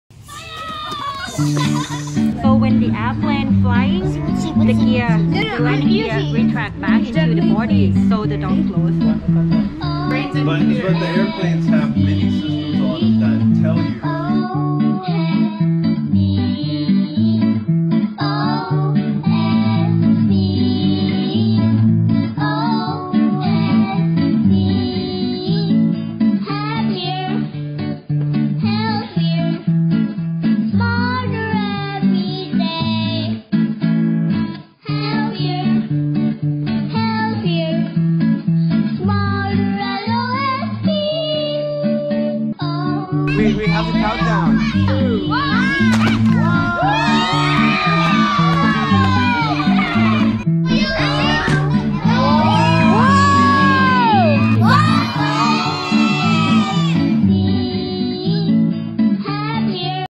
Outdoor School Bangkok (OSB) - A visit by Boeing 2020
The kids were so inspired by the Boeing team last week, the rocket launch turned out to be a very exciting highlight.  Can you see how high our rockets went?